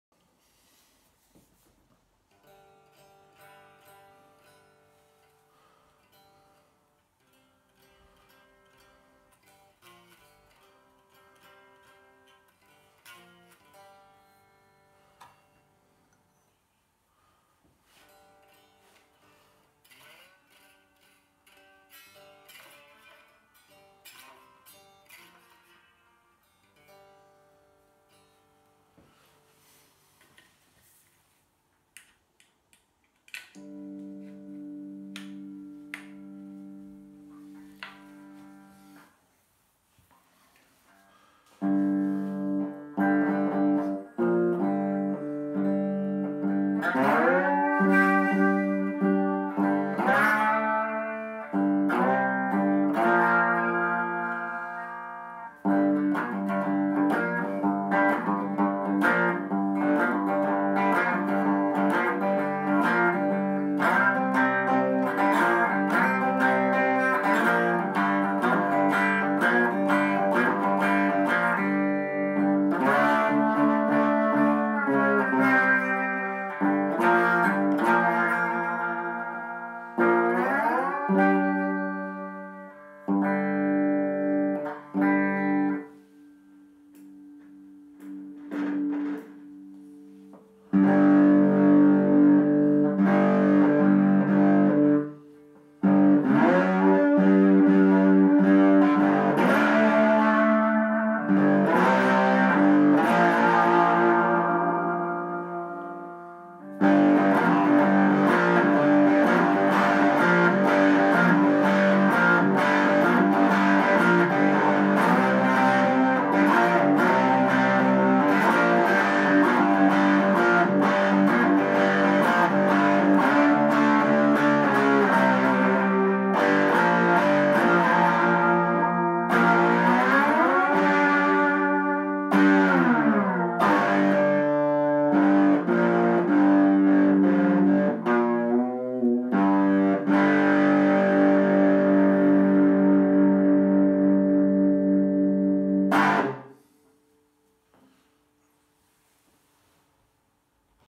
Three String Pick Axe Handle Guitar Soundcheck
Sound Check of Three String Pick Axe Handle Guitar
Tuned GDg